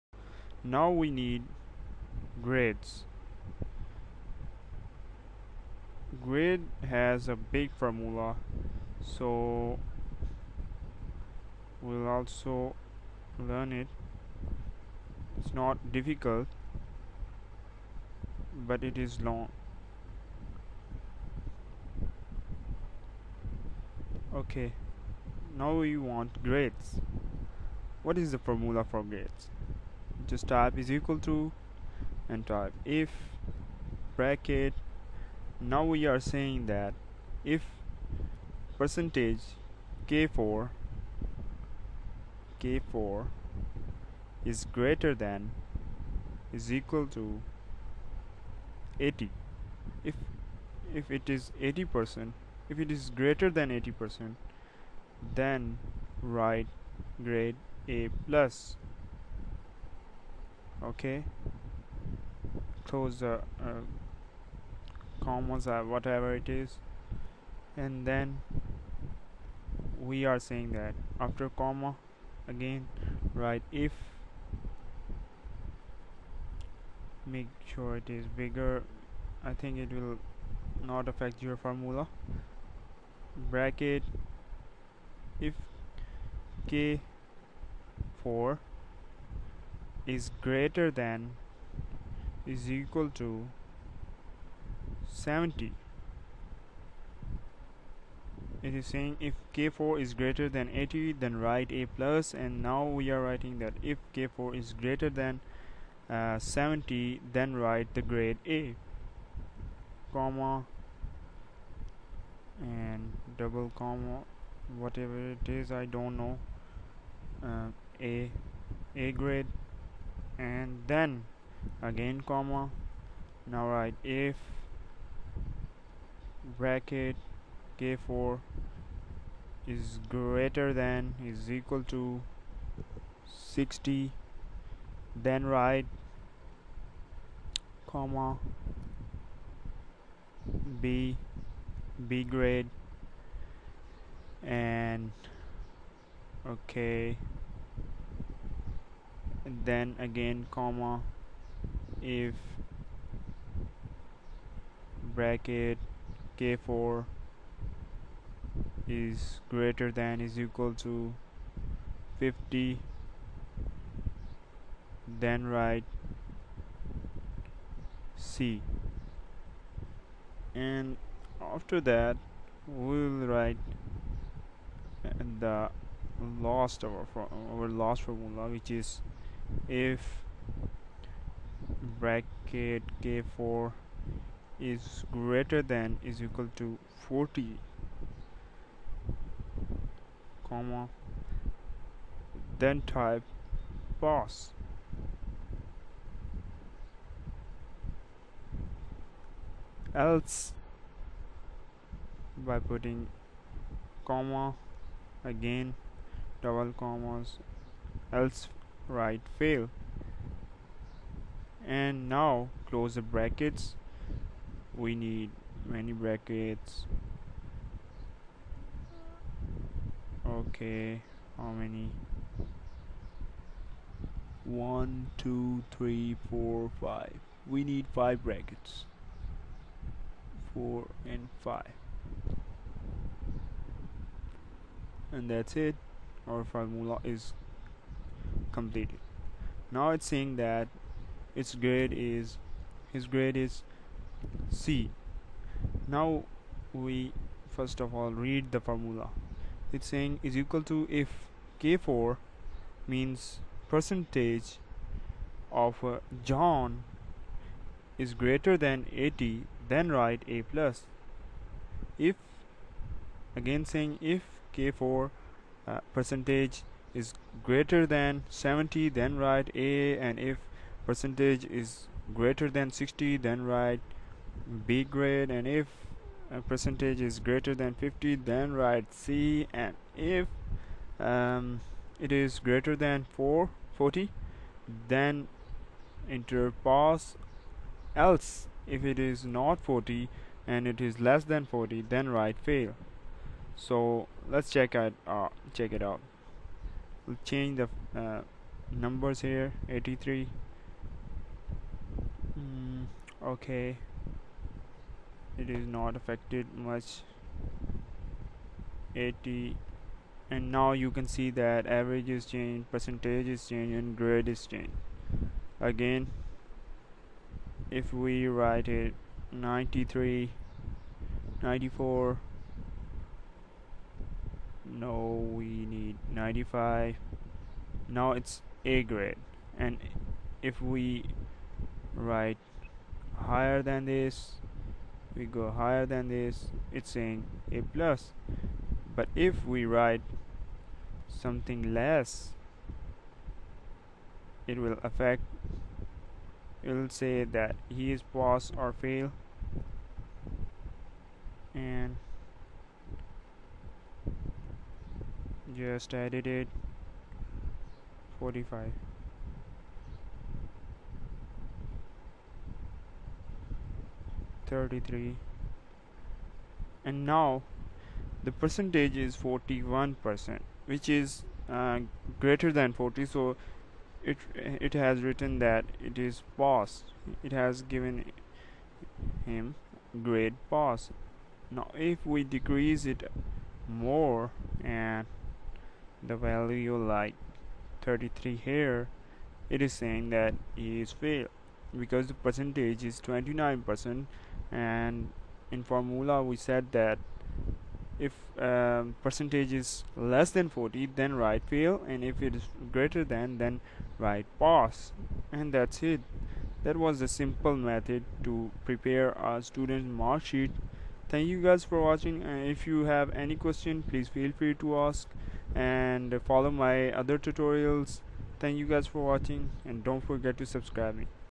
Making Students Marks Sheet in MS Excel 2013 - Part 3
Preparing Students Marks Sheet in MS Excel 2013
In this tutorial we will calculate
Obtain Marks
Average Marks
Percentage
Grades